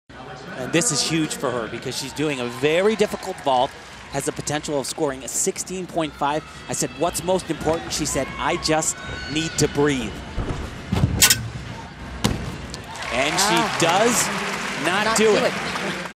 Alexandra Raisman - Vault - 2011 Visa Championships - Women - Day 1
Score: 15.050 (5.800, 9.250) - Aug. 18, 2011 - Xcel Energy Center - Saint Paul, Minn.